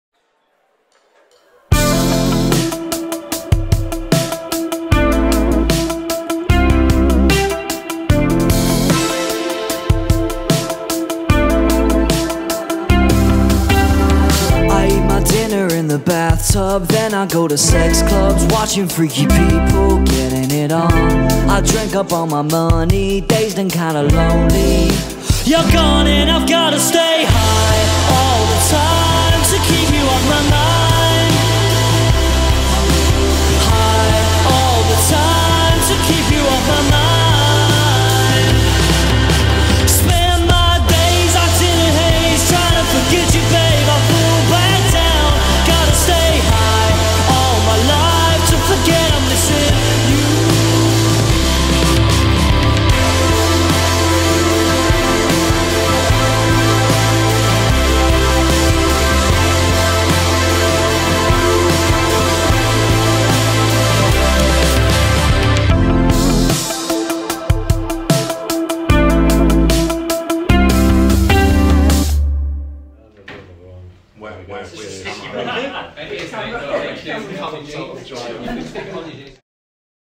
WATERCOLOURS // LEICESTER // DE MONTFORT HALL
27/04/2019 - Watercolours supporting Marillion at Leicester's De Montfort Hall.